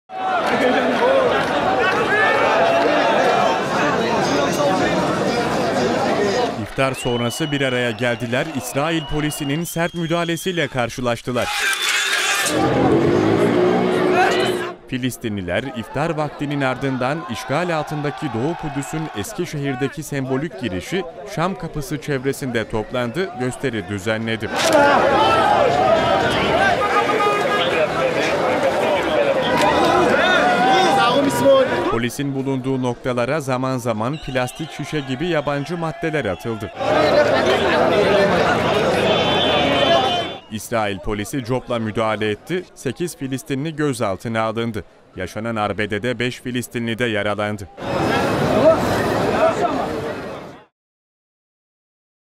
İftar Sonrası bir araya geldiler İsrail polisinin sert müdahalesi ile karşılaştılar!
Filistinliler, Şam kapısında toplandı gösteri düzenledi. Polisin bulunduğu noktalara plastik şişe gibi yabancı maddeler atıldı. İsrail polisi copla müdahale etti 8 kişi gözaltına alındı. Olaylarda 5 Filistinli de yaralandı.

TÜRKSAT 4A
Frekans: 12729
Sembol: 30000
Polarizasyon: V (Dikey)